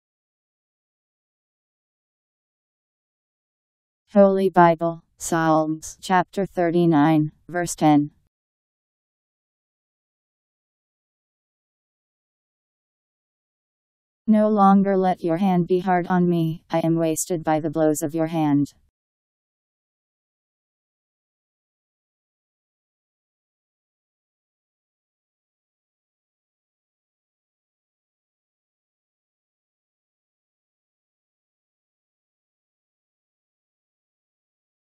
Holy Bible Psalms 39:10
Video shows Holy Bible Psalms Chapter 39 Verse 10: No longer let your hand be hard on me; I am wasted by the blows of your hand. Powered by MaryTTS. Text taken from Bible in Basic Edition. Bible Psalms 39:10